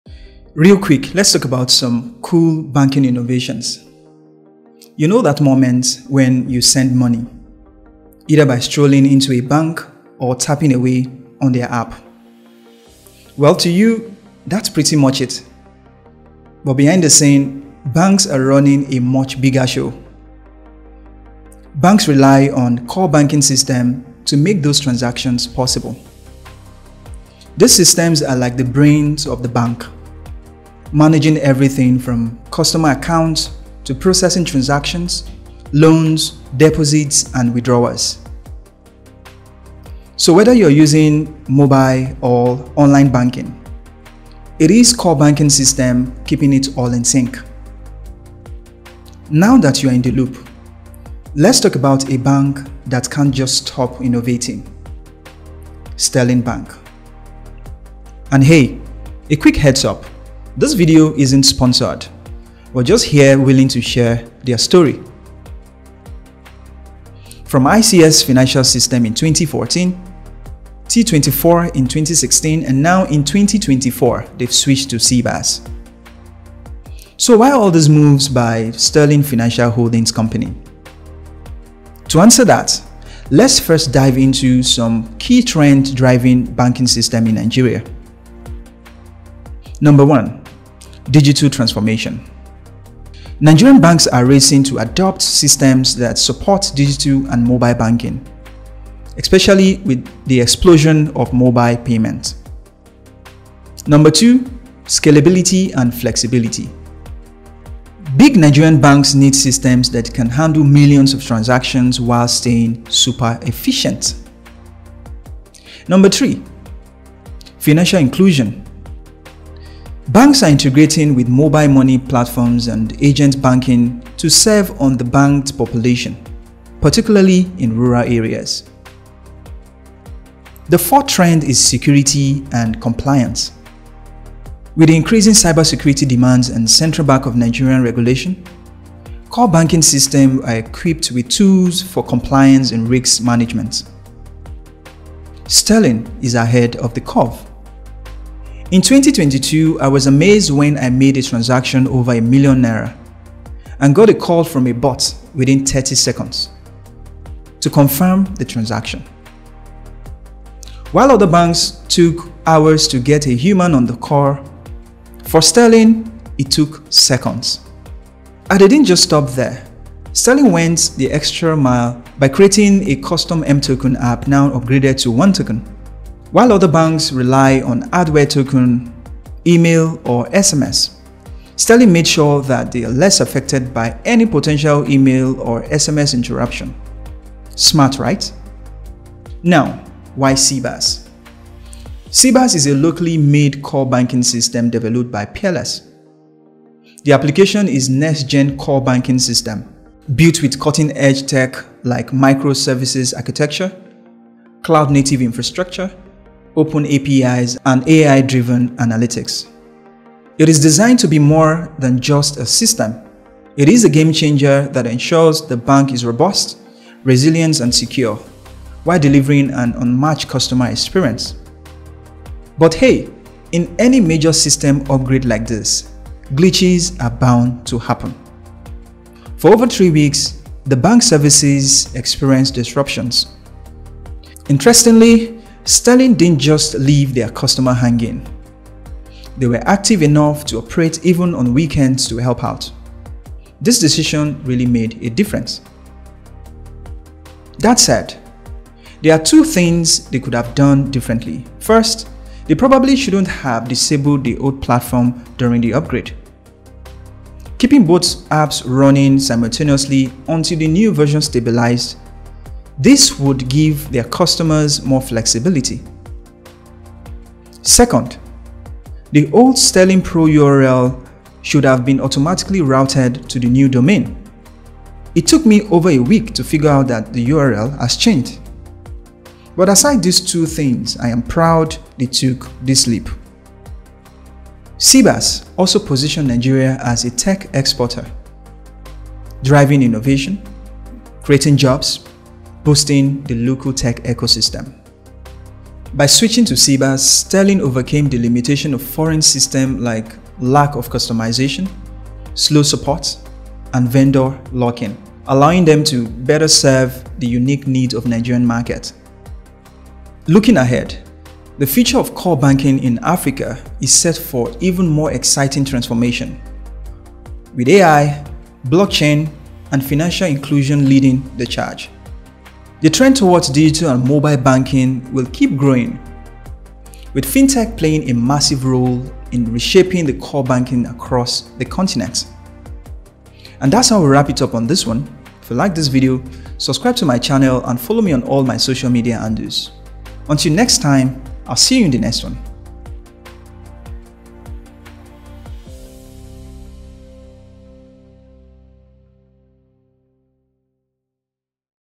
Sterling Bank's Bold Move to SeaBaaS: Revolutionizing Core Banking in Nigeria
In this video, we dive into how Sterling Bank is transforming the banking landscape in Nigeria with their latest upgrade to SeaBaaS, a cutting-edge core banking system. Discover why Sterling made the switch from ICS Financial System and Temenos T24 to SeaBaaS, and how this move addresses key trends in the Nigerian banking sector, including digital transformation, scalability, financial inclusion, and enhanced security.

We’ll explore the innovative features of SeaBaaS, such as its use of microservices architecture, cloud-native infrastructure, and AI-driven analytics. Plus, we’ll discuss the challenges faced during the upgrade process, including system disruptions and URL changes, and how Sterling Bank tackled these issues to maintain customer service.

Whether you're a banking enthusiast or just curious about the future of financial technology in Africa, this video offers insights into how SeaBaaS is setting new standards for core banking systems and driving innovation in the region.